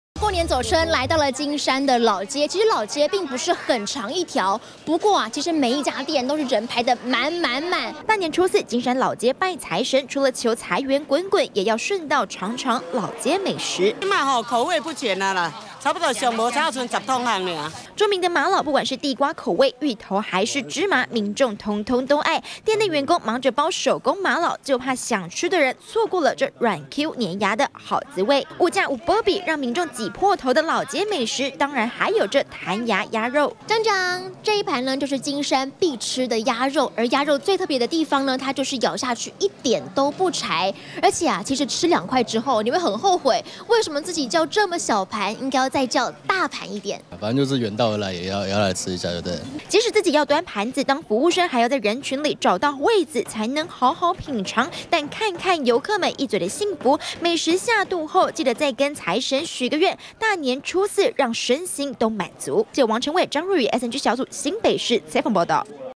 20150222中天新聞 18種口味麻荖、鴨肉 金山老街必吃
大年初四迎財神，金山財神廟被民眾擠得水洩不通，不少民眾求財前，先到老街填飽肚子，像是麻荖還有鴨肉，都是金山必嚐名單之一。